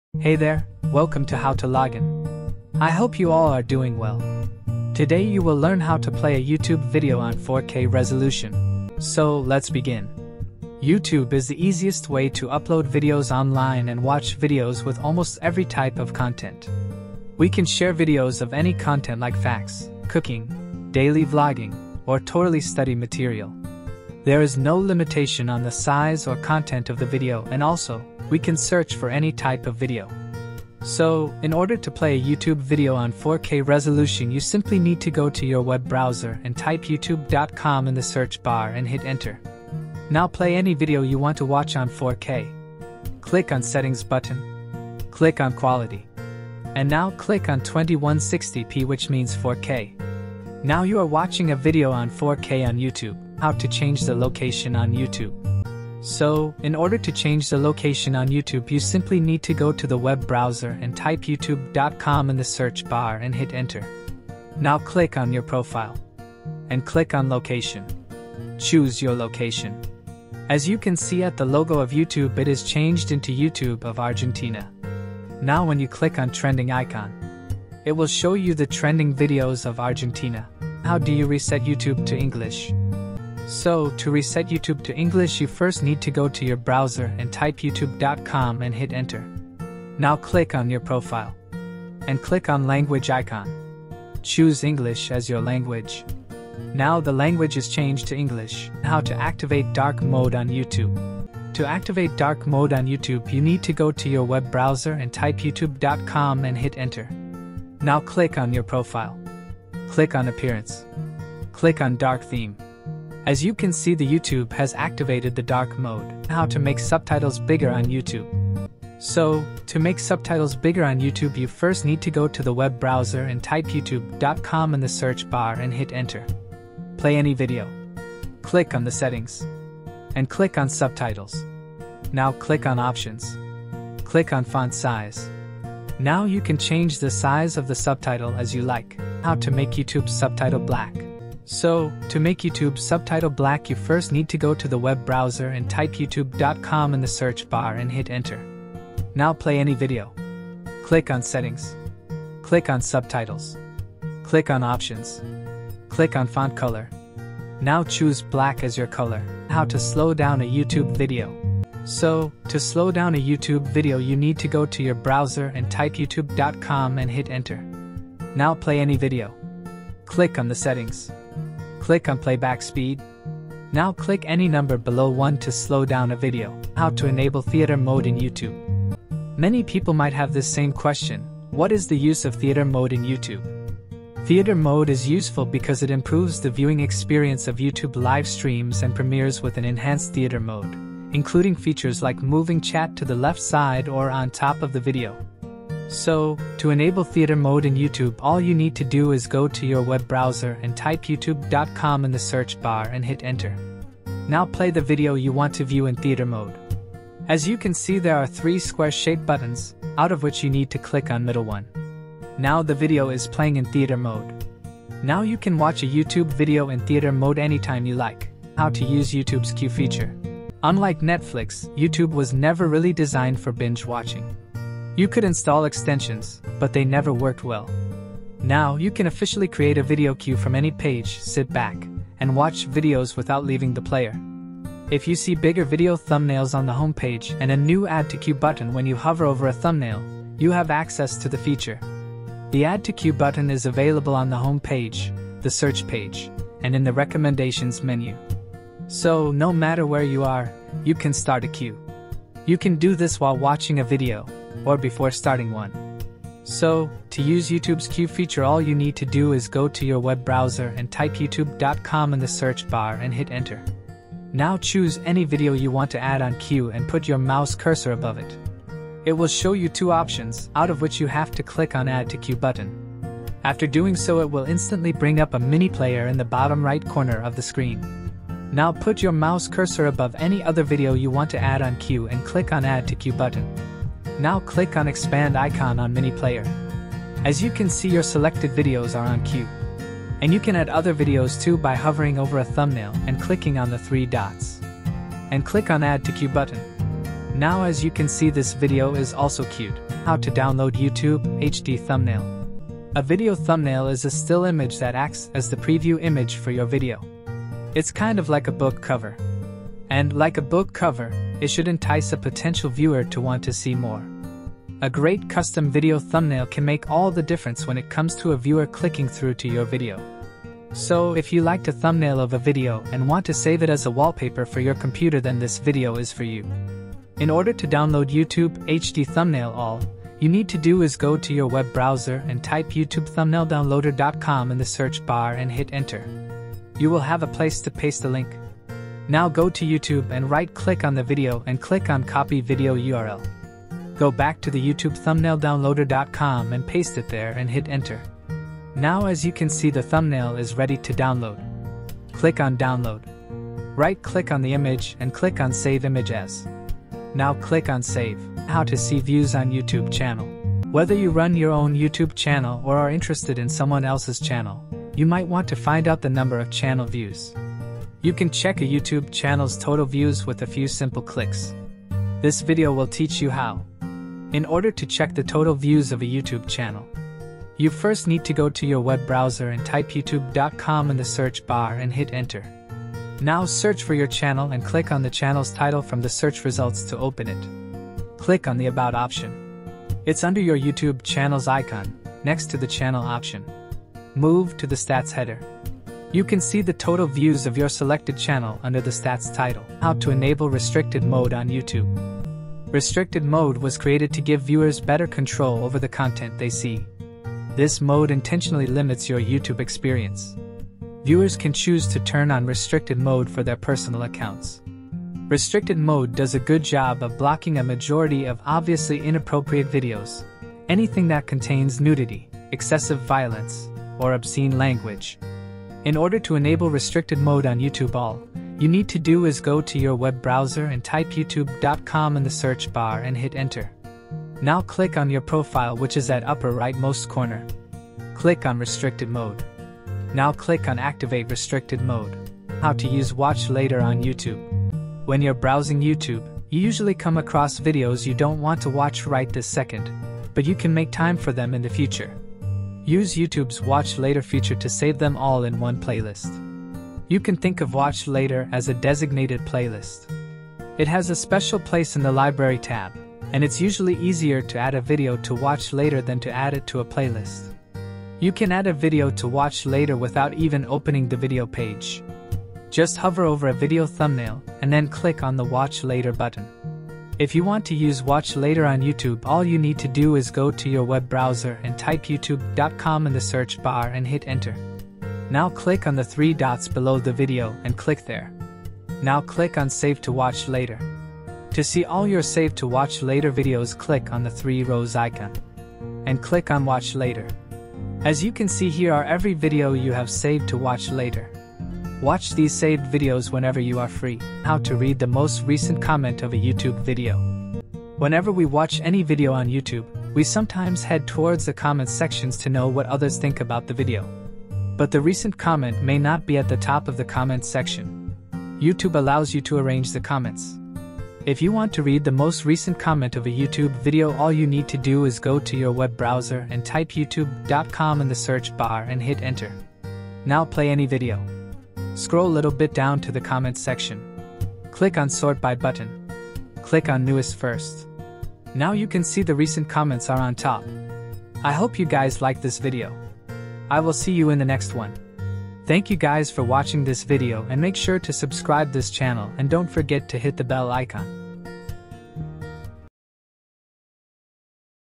How to Use YouTube on Desktop PC 2023? Youtube Tutorial for Beginners
How to Use Youtube for beginners? (Ultimate Guide)

Welcome to our comprehensive beginner's guide to using YouTube! If you're new to the platform, fear not – we're here to walk you through the process step by step. Starting with signing up, we'll show you how to navigate the viewing experience and offer some extra tips to make your journey even smoother.

With this guide, you're fully equipped to embark on your YouTube journey. From signing up to navigating videos and beyond, you're all set to explore the vast realm of YouTube content. Happy watching! 📺🎉

Youtube Tutorials on:

0:06 play youtube on 4k
0:53 Change Youtube location
1:20 Reset youtube language to english
1:38 Activate dark mode on youtube
1:56 Make youtube subtitles bigger
2:19 make youtube subtitle black
2:39 Slow down youtube video
2:56 Enable youtube theater mode
3:38 use youtube queue feature
5:00 download youtube HD thumbnail
5:58 see views on Youtube
6:39 enable youtube restricted mode
7:22 use Youtube watch later option
8:24 YouTube most recent comment